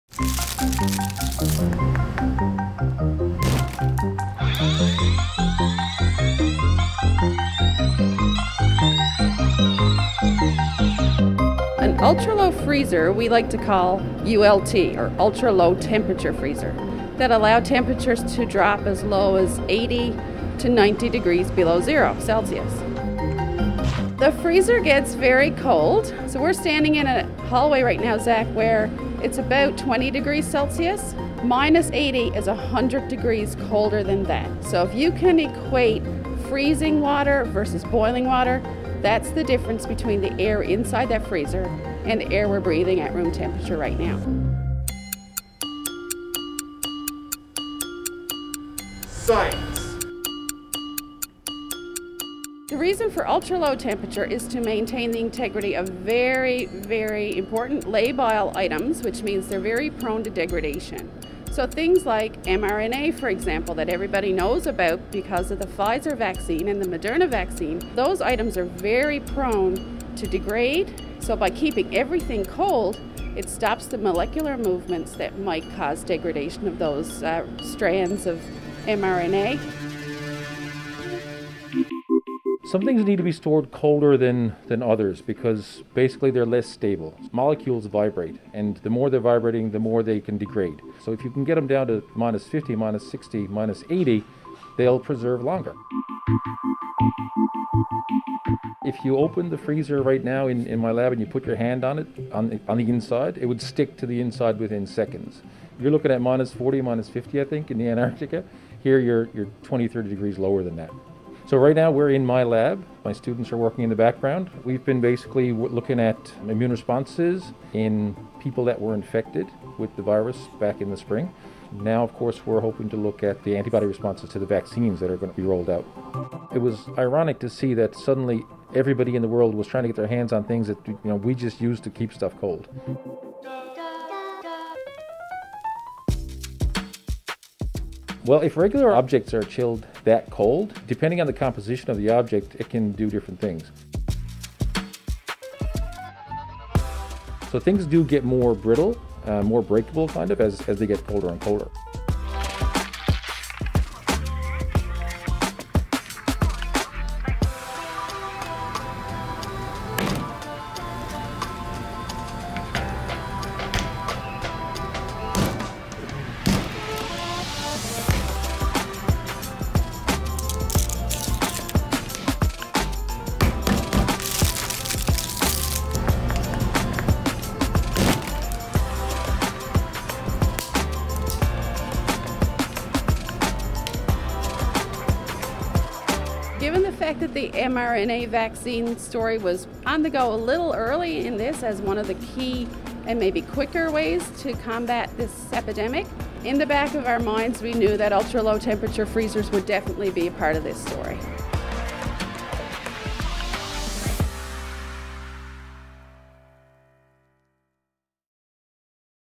We brrr-ing you inside an ultra-cold freezer used for the COVID-19 vaccine
Ultra cold freezers can keep vaccines for COVID-19 chilled to sub-Arctic temperatures. But what happens if you put food from a regular fridge inside? Read our full story